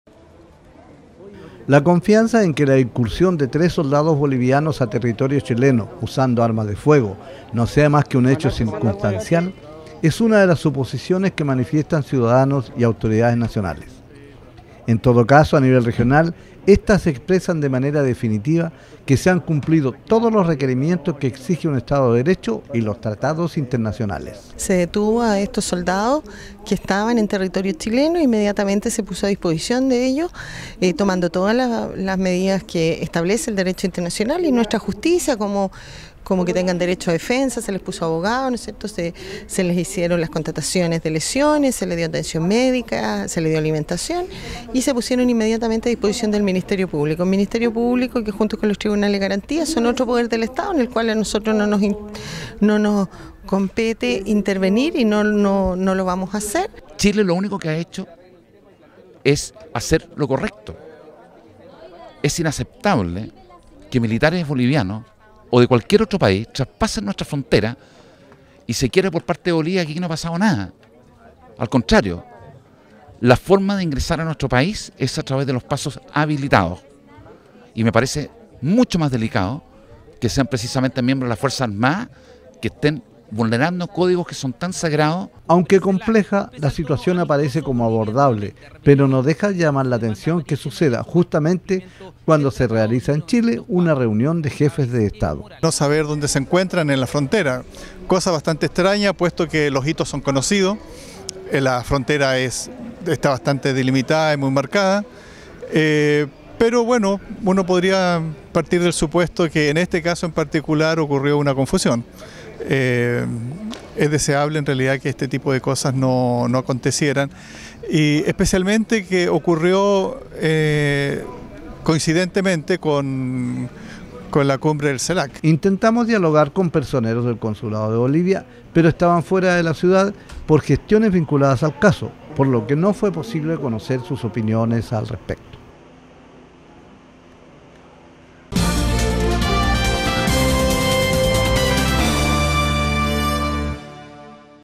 DERECHO Y BUENA FE DEBE RESOLVER CASO DE SOLDADOS BOLIVIANOS - Iquique TV
(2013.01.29)  Presumir que la incursión de tres soldados bolivianos a territorio chileno fue un hecho circunstancial, es una de las alternativas que se visualizan en torno al incidente que los llevó a ser detenidos, por uso de arma de fuego, en la zona de Colchane.
La reacción de Bolivia parece desmedida cuando condecora a los soldados, en acto reiterado, tras cruzar a Chile de modo, al menos, irregular. En especial, cuando los canales regulares dieron a conocer oficialmente de la situación al gobierno altiplánico, a través de las respectivas cancillerías.